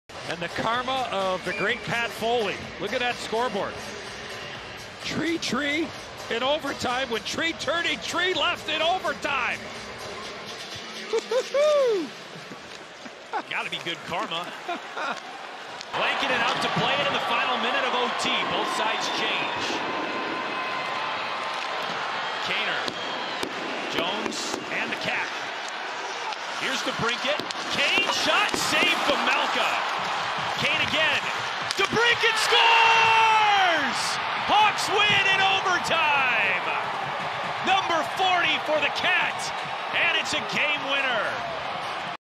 Pat Foley Karma Gives Blackhawks OT Win?
Eddie Olczyk thinks his great former TV broadcast partner might be sending the Blackhawks some positive overtime juju over the Arizona Coyotes on April 20, 2022.